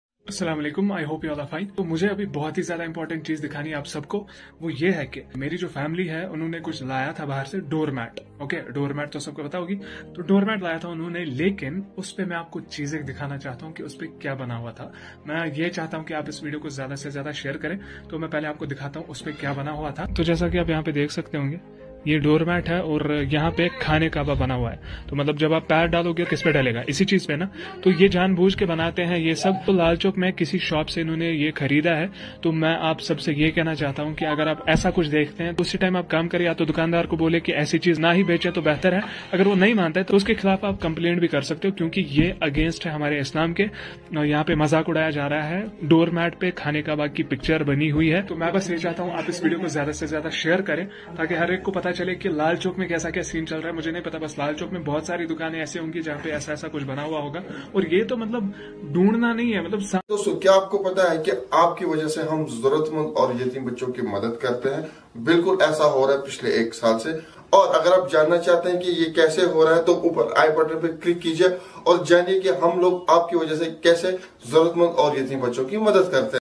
Astagfirullah 😭😭 Extremely Shocking News | Khana Kaaba Picture Made on Door Mats | Please Share it
Kashmir Extremely Shocking News | Kashmiri Muslims Please Share it | Khana Kaaba Made on Door Mats
Camera's Used:
Canon 200D Mark II
Mic Used:
BOYA BY M1 Omnidirectional Lavalier Microphone
Tripod Used:
Benro T-880EX